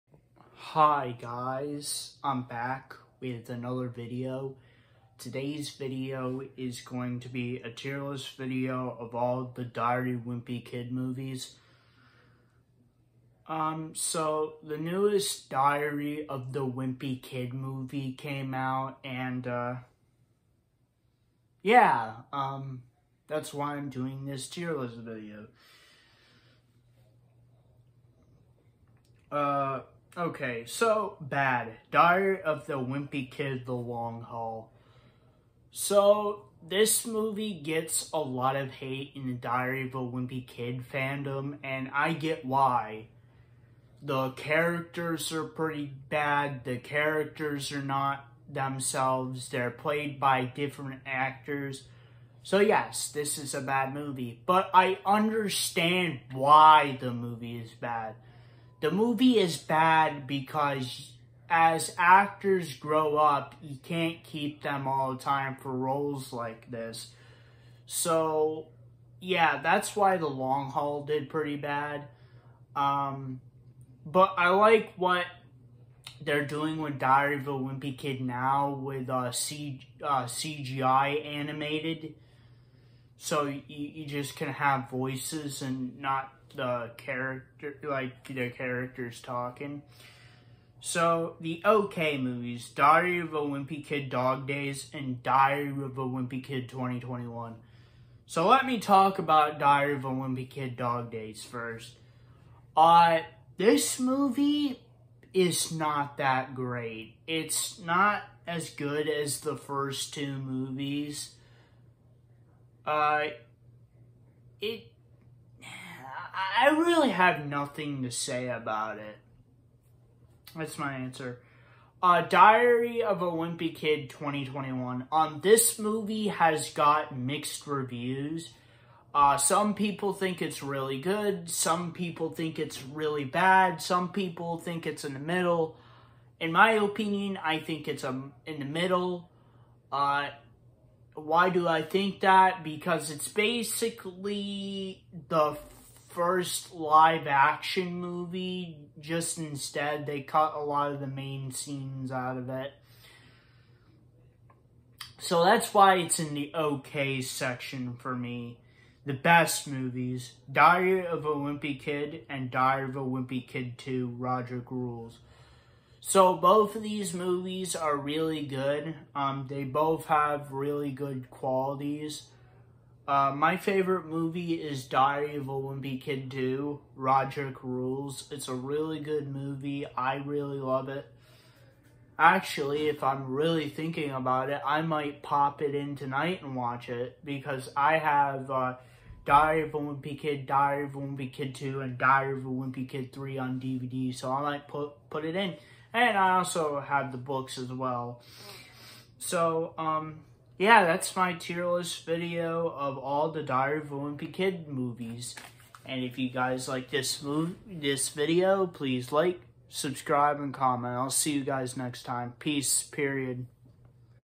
Tier list video of all the diary of a wimpy kid movies
Hi guys I’m back with another video so today’s video is going to be a tier list video of all the diary of a wimpy kid movies so I’m doing this because The newest diary of a wimpy kid movie came out 2021 the CGI animated 2021 diary of a wimpy kid movie came out of course diary for wimpy kid it’s a part of my childhood I have pretty much have everyone except the newest book and the live action movies were definitely a part of my childhood I even mentioned that I might put in Rodrick rules later and watch it like you guys know how my tier list goes already this one is rated from Bad to best so that’s my tier list video of all the diary of a wimpy kid movies if you guys like this Video please like subscribe and comment I’ll see you guys next time Peace.￼